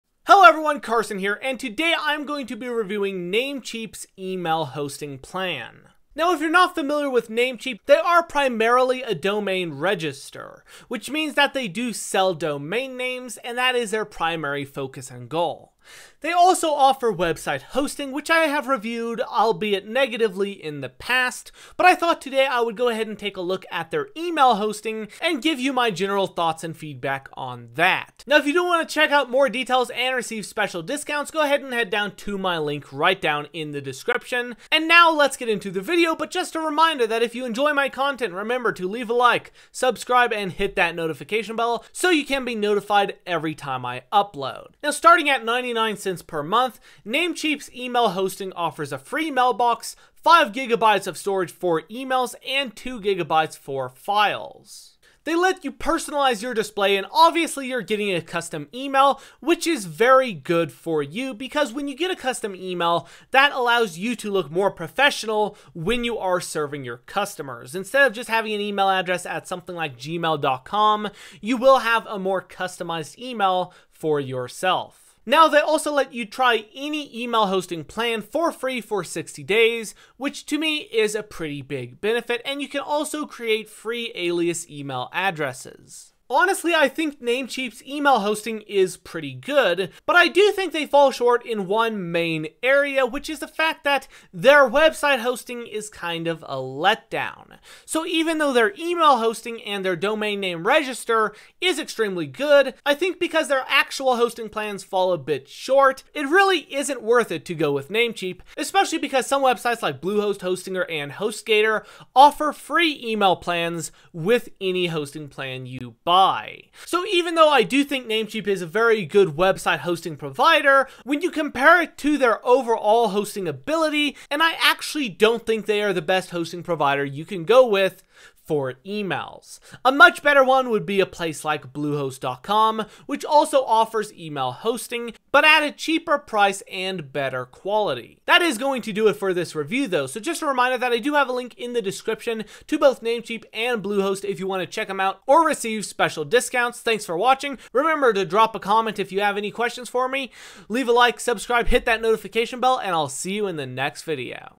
Best Small Business Email Hosting 🔥 Namecheap Email Hosting Review
---Timestamps---
00:00 Intro
00:07 What is Namecheap Email hosting
01:28 Downside of Namecheap
02:09 Better Alternatives
02:18 Outro

Hello everyone Carson here and today I'm going to be reviewing named cheeps email hosting plan. Now if you're not familiar with name cheap, they are primarily a domain register, which means that they do sell domain names and that is their primary focus on goal. They also offer website hosting, which I have reviewed, I'll be at negatively in the past, but I thought today, I would go ahead and take a look at their email hosting and give you my general thoughts and feedback on that. Now if you don't want to check out more details and receive special discounts go ahead and head down to my link right down in the description. And now let's get into the video.

Now starting at 99 cents per month, named chiefs, email hosting offers a free mailbox, five gigabytes of storage for emails and two gigabytes for files. They let you personalize your display. And obviously you're getting a custom email, which is very good for you. Because when you get a custom email that allows you to look more professional when you are serving your customers. Now they also let you try any email hosting plan for free for 60 days, which to me is a pretty big benefit. And you can also create free alias email addresses.

Honestly, I think name cheeps email hosting is pretty good. But I do think they fall short in one main area, which is the fact that their website hosting is kind of a letdown. So even though their email hosting and their domain name register is extremely good. I think because their actual hosting plans fall a bit short. It really isn't worth it to go with name cheap, especially because some websites like Bluehost hosting her and Hostgator offer free email plans with any hosting plan you buy.

So even though I do think Namecheap is a very good website hosting provider when you compare it to their overall hosting ability, and I actually don't think they are the best hosting provider you can go with. For emails a much better one would be a place like Bluehost calm, which also offers email hosting, but at a cheaper price and better quality that is going to do it for this review though. Remember to drop a comment if you have any questions for me.